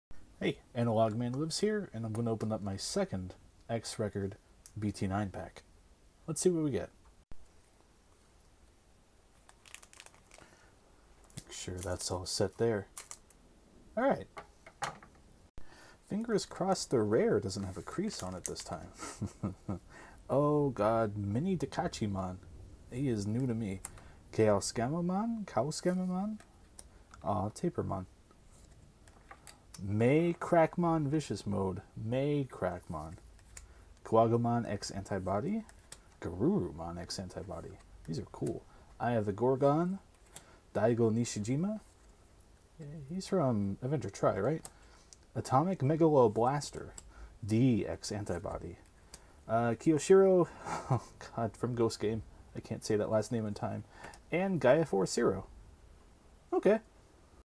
Opening my second X Record BT09 Digimon TCG pack!
Went to Walmart and grabbed a couple packs! Before this I've only bought a pack of BT05 and a couple singles off eBay so I'm very new to the Digimon TCG. How'd I do?